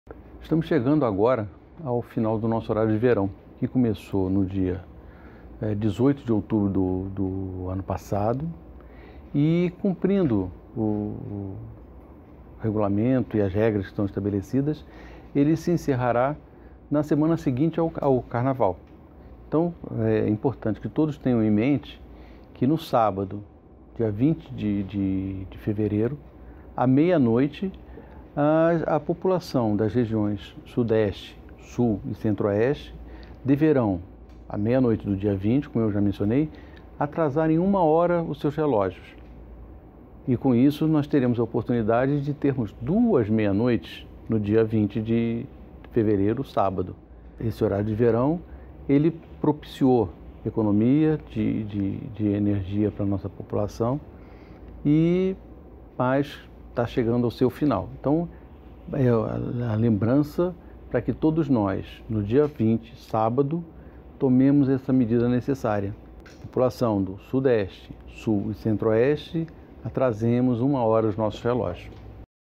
Horário de verão termina neste sábado
ESPECIAL WEB - 19.02.16 Neste sábado, à meia noite, acaba o horário de verão. Quem mora nas regiões Centro-Oeste, Sudeste e Sul deve atrasar o relógio em uma hora. O secretário-executivo do Ministério de Minas e Energia, Luiz Eduardo Barata, fala sobre a economia de energia e lembra a população sobre o fim do horário de verão e a necessidade de acertar o relógio.